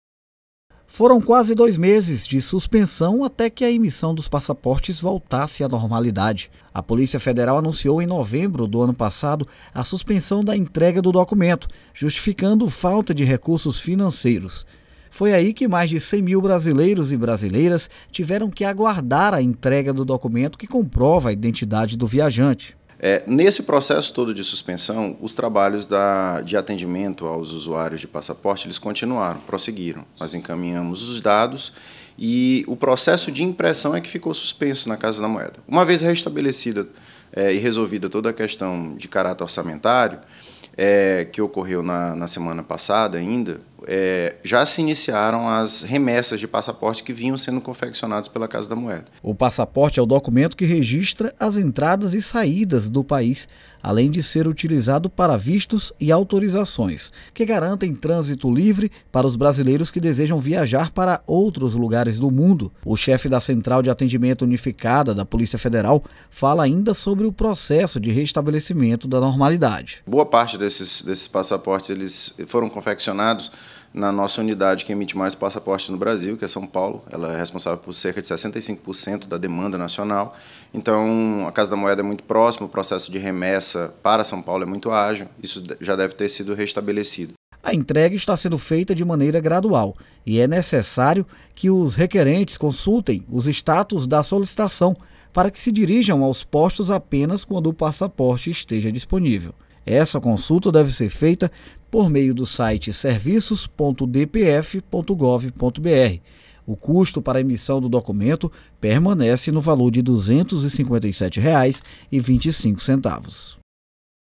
Passaportes voltam a ser emitidos pela Polícia Federal após quase dois meses de serviço suspenso
A Polícia Federal anunciou a volta da emissão dos passaportes, documento que registra as entradas e saídas do país, além de ser utilizado para vistos e autorizações, que garantem trânsito livre para os brasileiros que desejam viajar para outros lugares do mundo.
A entrega está sendo feita de maneira gradual após quase sessenta dias de suspensão por falta de recurso financeiro.